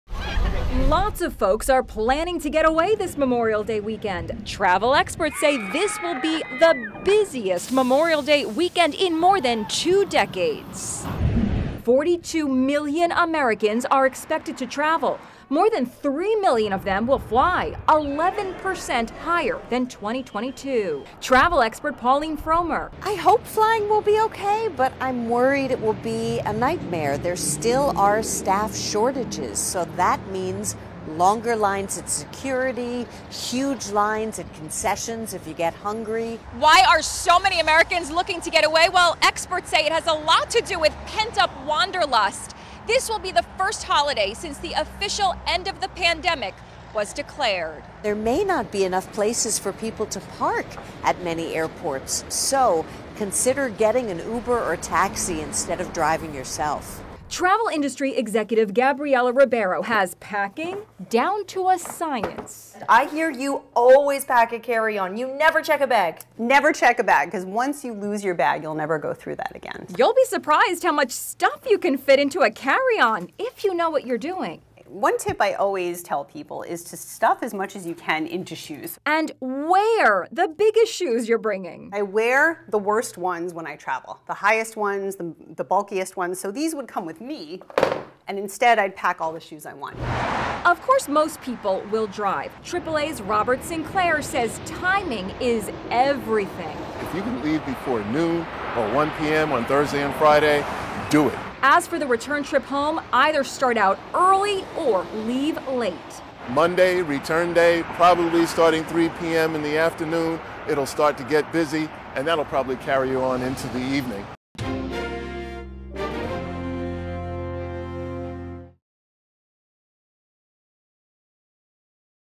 Will This Memorial Day Weekend Be the Busiest in Years?
Travel experts say this Memorial Day weekend will be the busiest in nearly 20 years. Forty-two million Americans are expected to travel and more than three million of them will fly to their destination. Experts say so many Americans are looking to get away because this will be the first holiday since the end of the pandemic was declared. Inside Edition's Ann Mercogliano has some tips on how to make your trip a breeze.